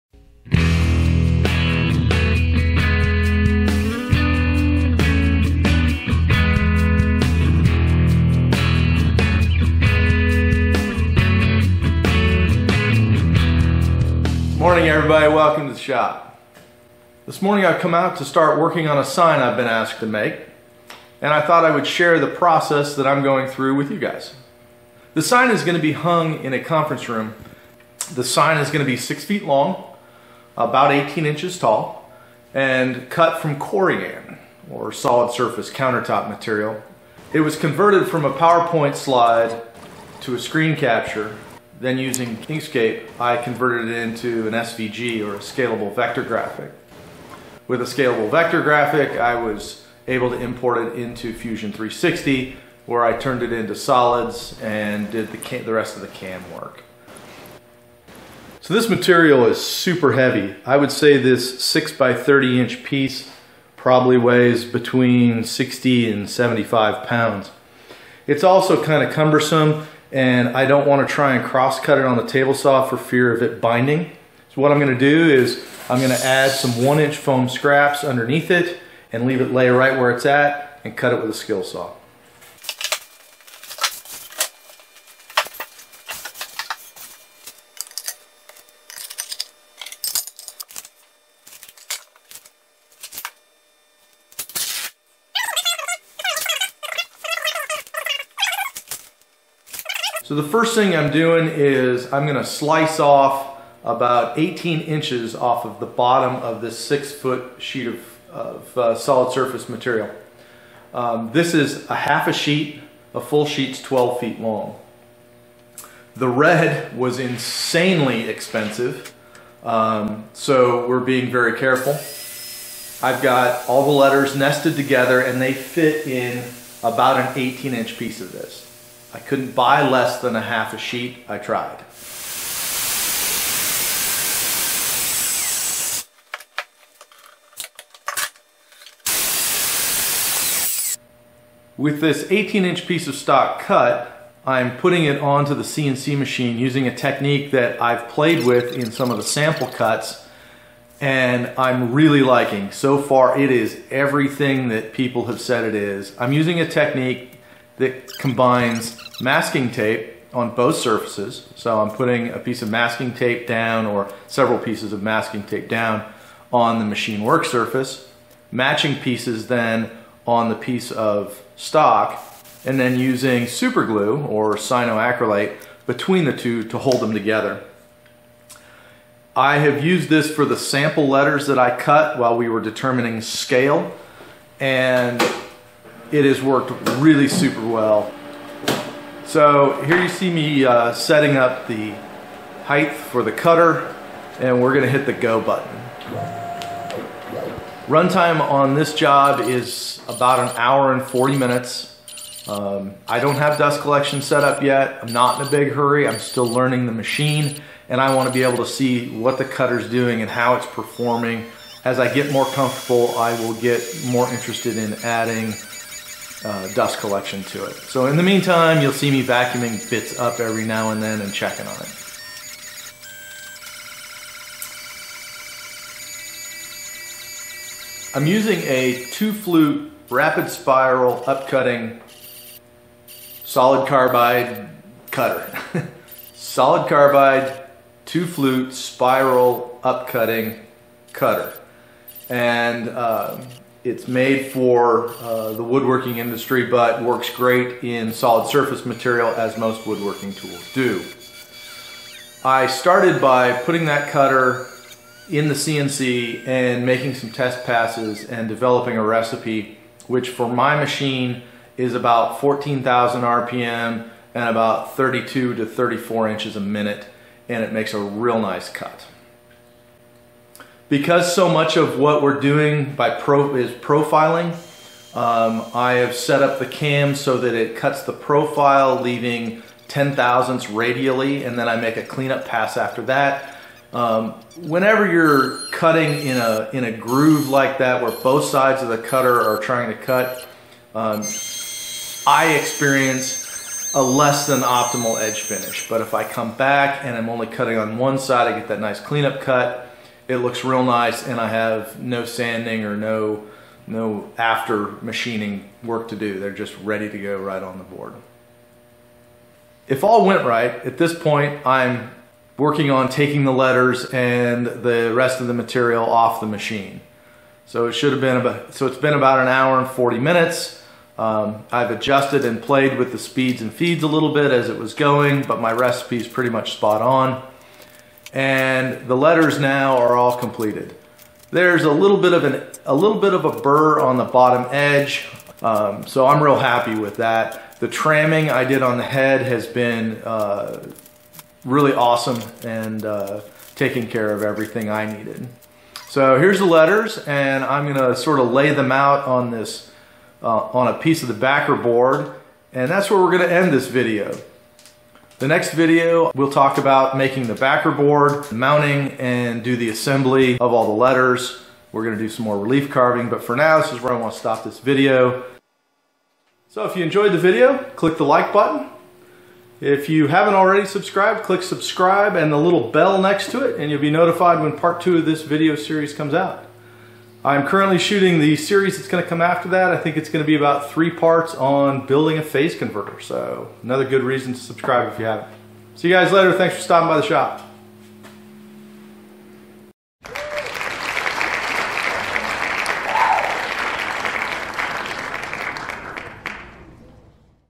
CNC Routed Solid Surface Sign
Part 1 of 2 in the making of a 6' sign out of solid surface material.  The completed sign has relief carved letters as well as applied letters that are profile cut.